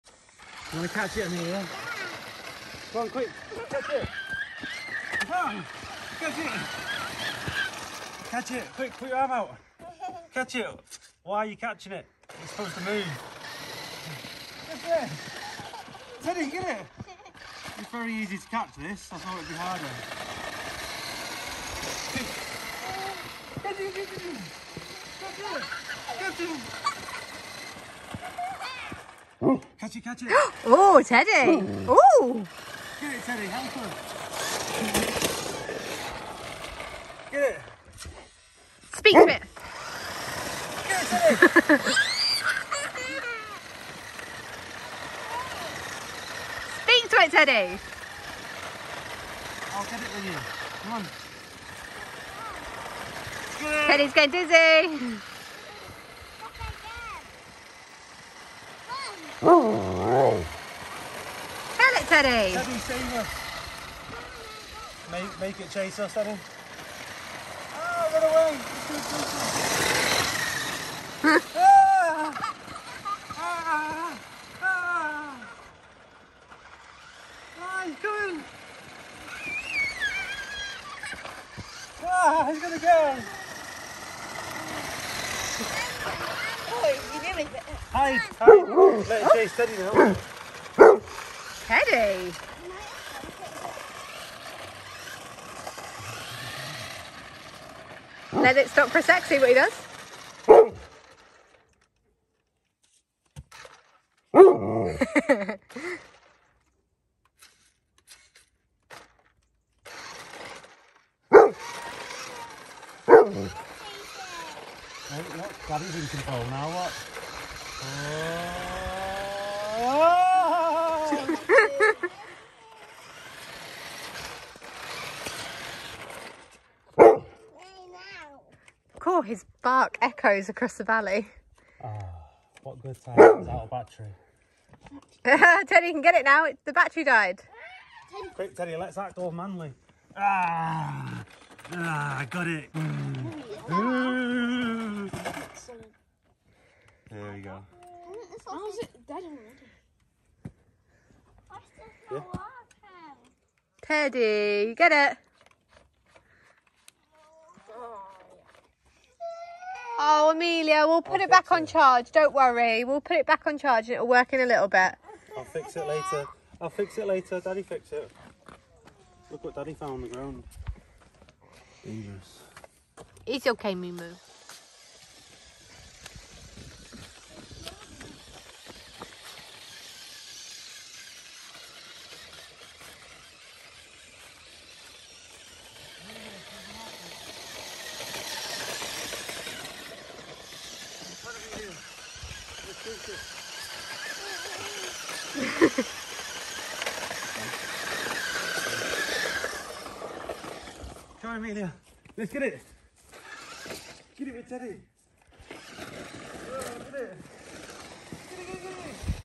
Giant Husky Argues With RC Toy Car! Amelia Thinks It's So Funny!
Teddy loves the chase! We stuck a little sausage in the trunk and kept him entertained for hours haha. He gets frustrated at the gadgets and it's so funny to watch him, Amelia loves playing with Dad